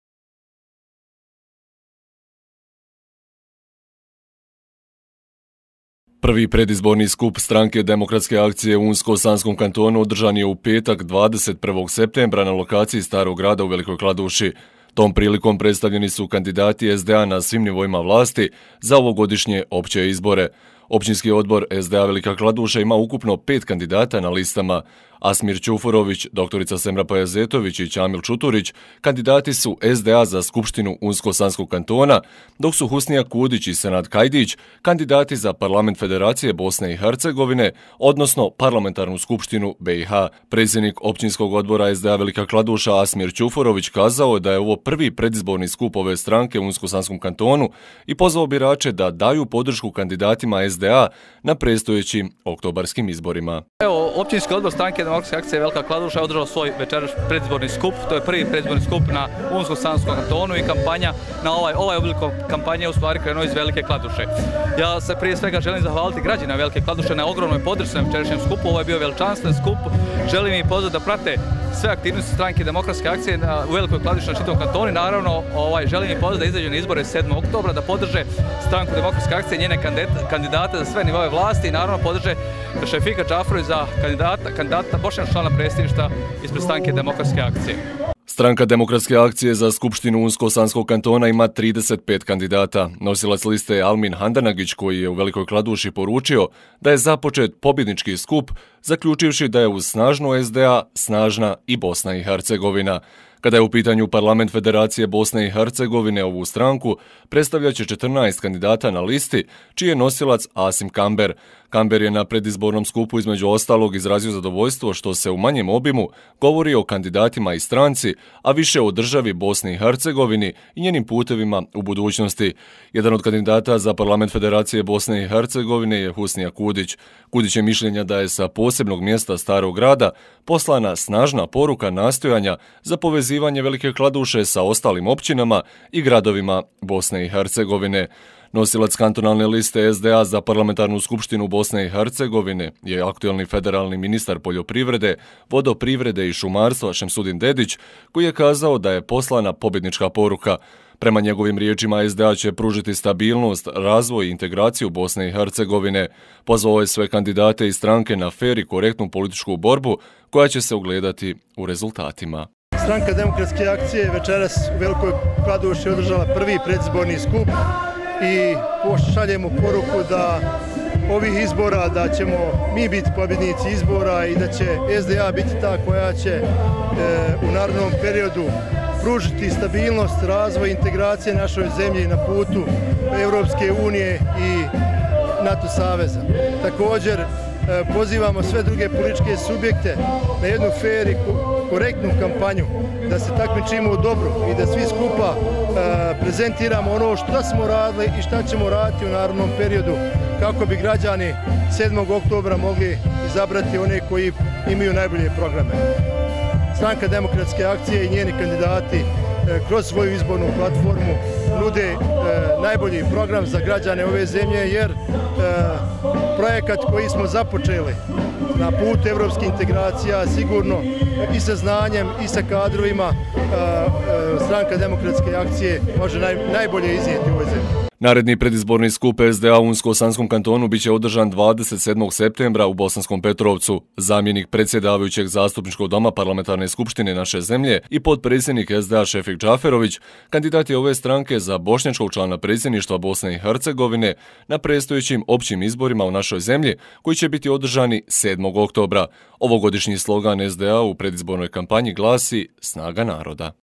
RVK - SDA u Velikoj Kladuši održala predizborni skup (21.09.2018.)
Prvi predizborni skup Stranke demokratske akcije (SDA) u Unsko-sanskom kantonu održan je u petak, 21. septembra na lokaciji Starog grada u Velikoj Kladuši. Tom prilikom predstavljeni su kandidati SDA na svim nivoima vlasti za ovogodišnje Opće izbore.